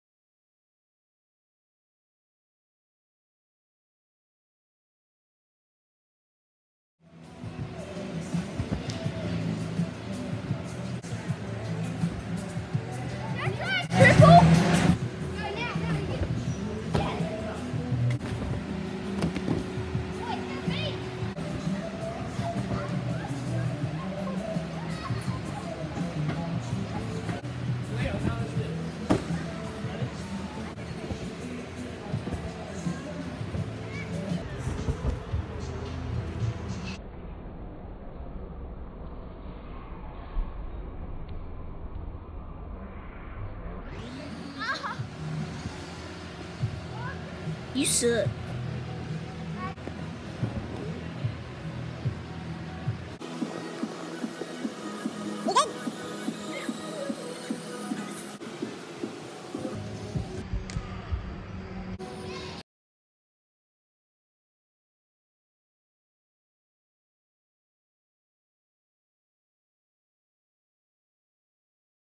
Trampoline session (pt 1) (spring loaded banora point)
Joey nearly broke or dislocated his thumb R.I.P Joey